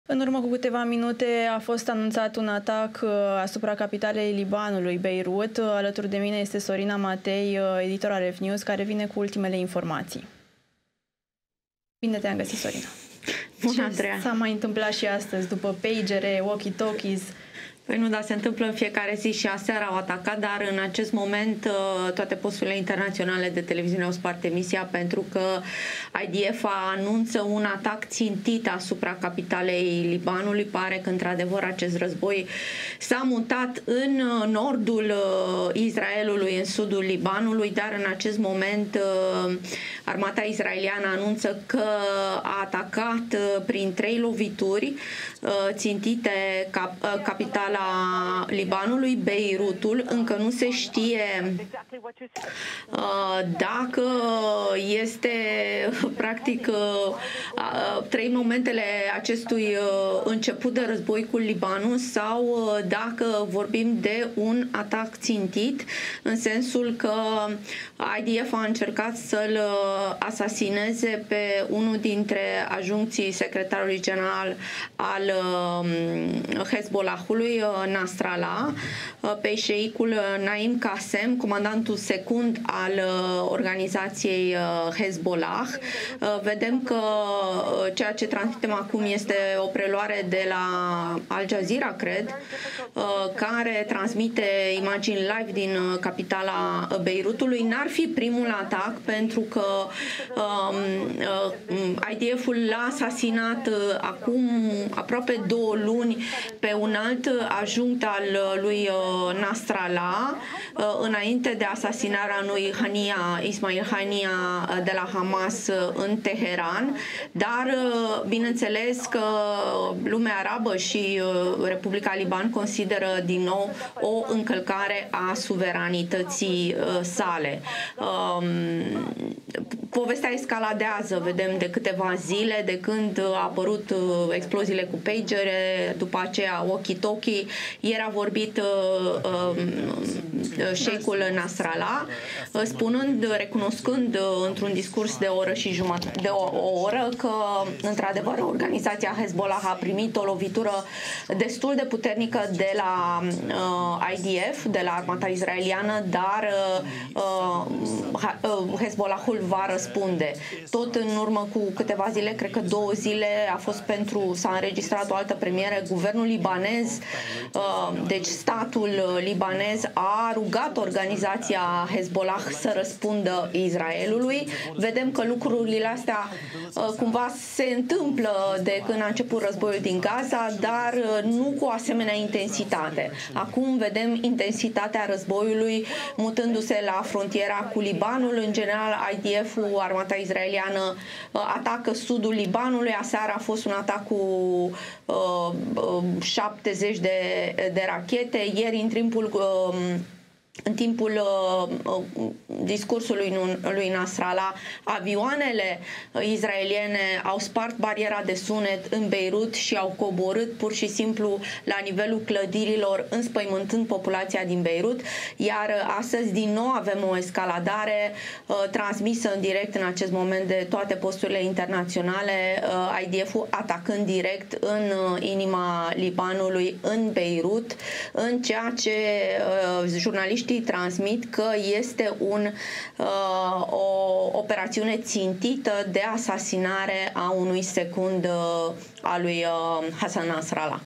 BREAKING NEWS: Armata israeliană atacă Beirut, capitala Libanului. Atacul l-a vizat pe Ibrahim Aqeel
➥ Armata israeliană a anunțat un atac asupra Beirutului, capitala Libanului.
➥ Israel a confirmat că atacul l-a vizat pe Ibrahim Aqeel, șef al Unității de Operațiuni Speciale a Hezbollah.
➥ Cel puțin 11 persoane au fost confirmate ucise, inclusiv femei și mai mulți copii.